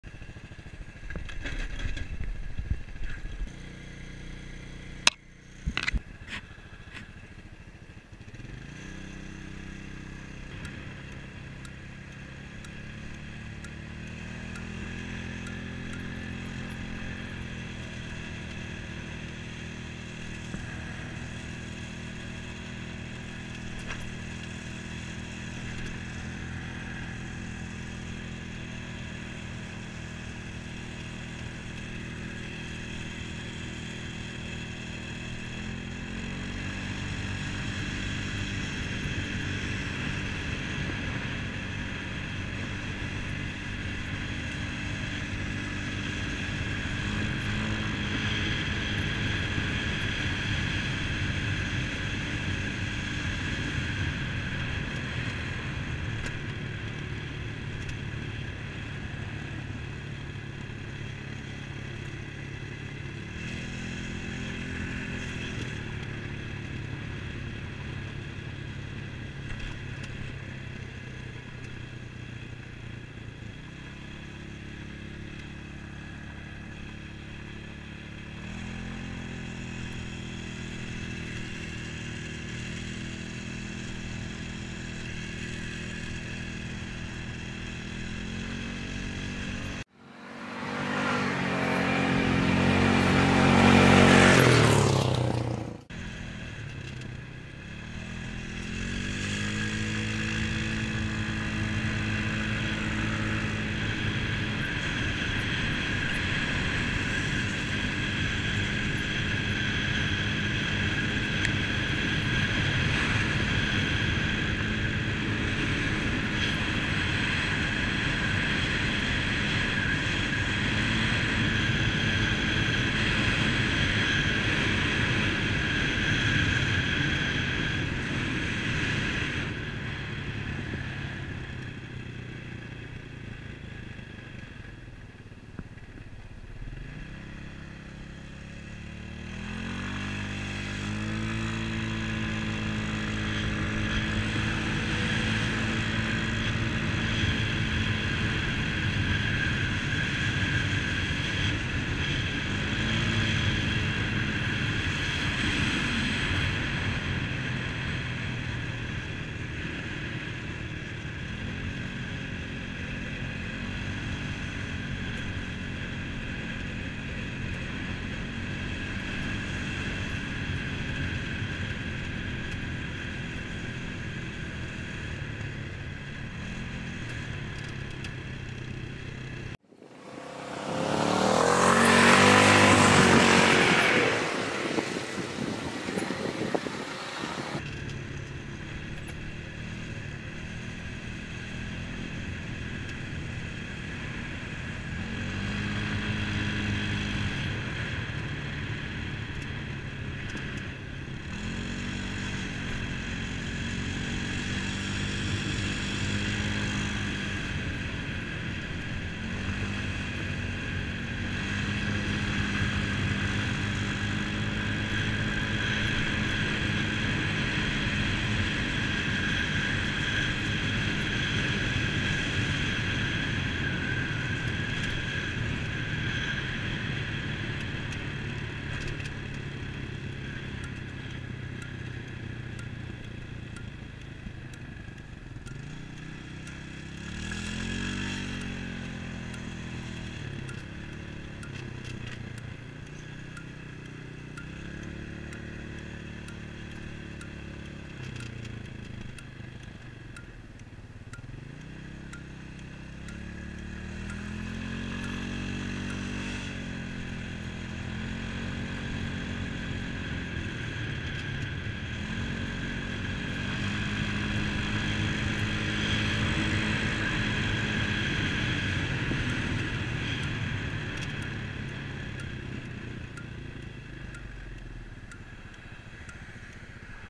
A gentle ride in the park
I really love my moped, I am very excited that the riding season is starting up again finally!

This video was taken from a couple different days in the same park on my Honda Elite 125cc moped :3